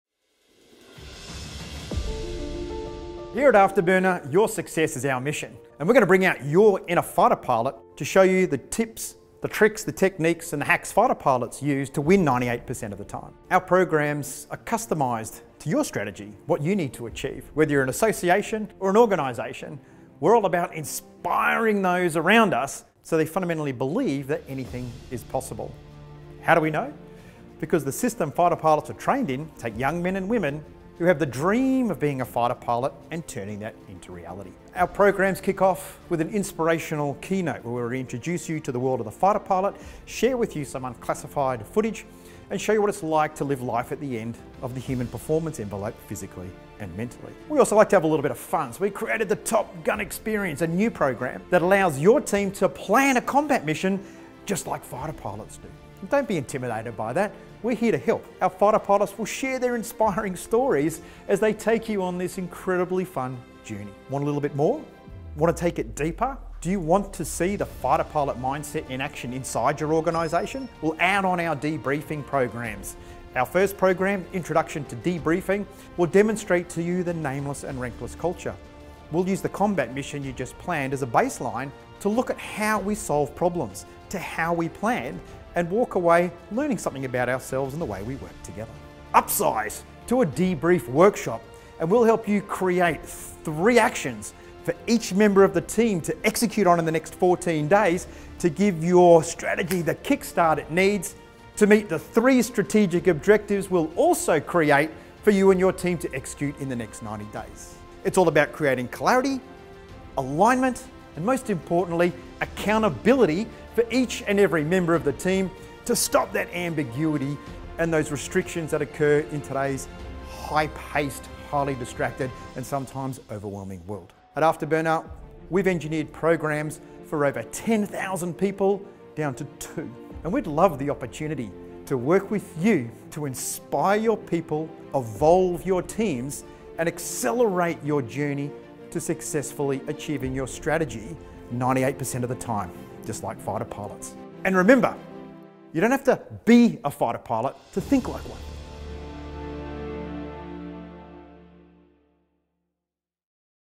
Watch this Summary to learn how Afterburner experiential programs can work for your team!
Afterburner experiences are tailored to your requirements, whether you only have a few hours for a keynote or a quick Top Gun unique team building experience, a half-day Afterburner learning program or a fully immersive two-day retreat!

Our CEO, Christian "Boo" Boucousis explains how the modular programs stand-alone or stack together to ensure your experience is a memorable one!

You'll experience the fighter pilot mindset, ways of working and our Flawless Execution methodology and the ability to consistently surpass expectations!